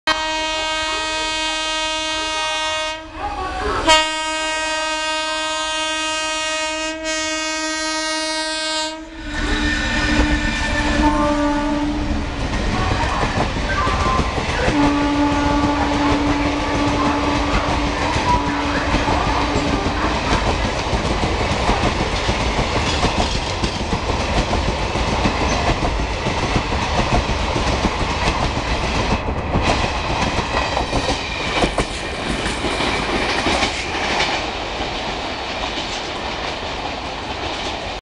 CNB WAG 7 ACCLERATES AT FULL THROTTLE WITH LALGOLA PASSENGER !!.
Date:31/12/2013

Kanpur (CNB) WAG 7 accelerates through Belgharia on Sealdah - Naihati section with all it's might (one can hear the whinning of the motors of the WAG 7) with 2 hours delayed Lalgola - Sealdah Passenger at it's tow.